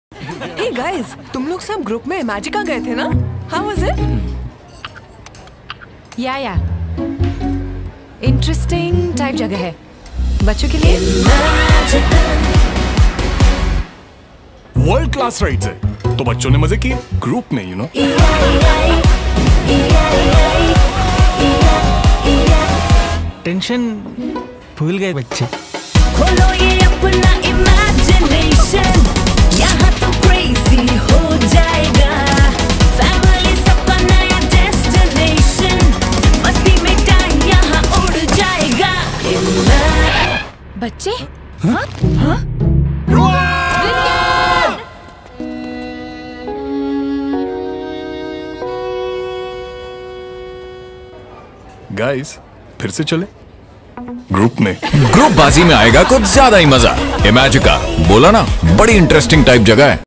Group Mein Hai Toh Mazaa Toh Aayega Groupbaazi at Imagica
The 360-degree promotional ad campaign by the theme park brand engages its target audience with the idea that fun multiplies with like-minded groups